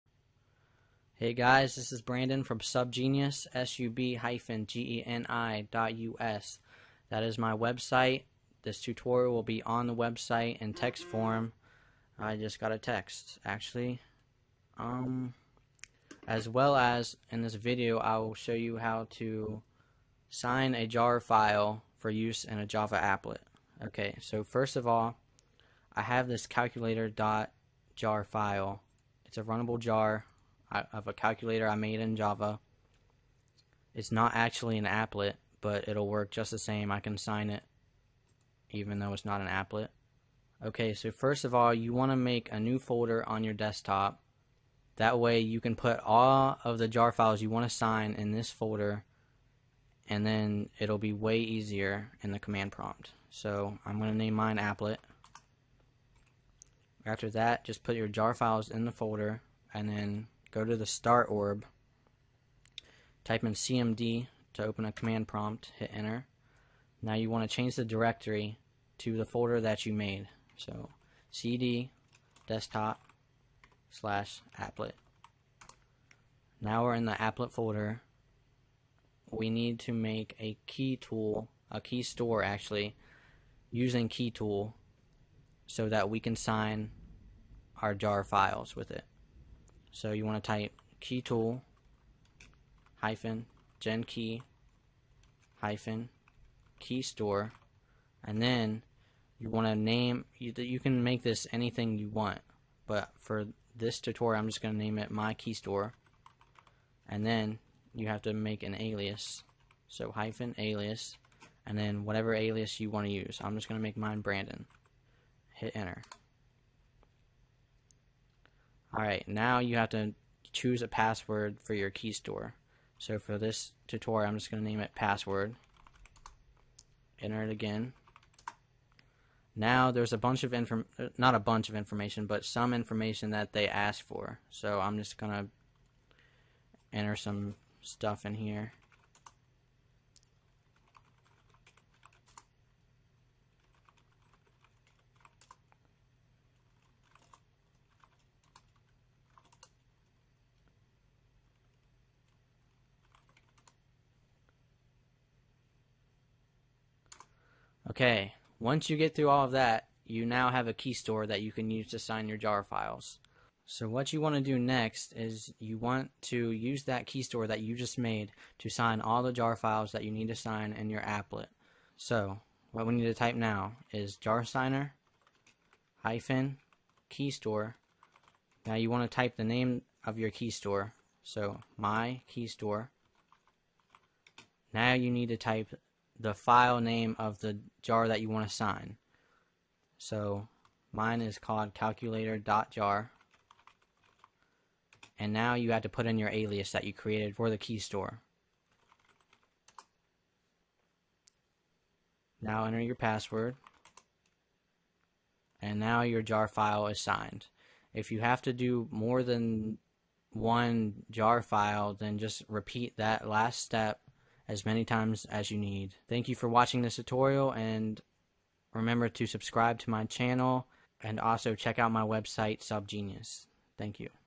How to Sign Jar Files for Applets - Java Tutorial
The text is easier to read in full screen with 720p

Signing jar files for java applets is easy mayne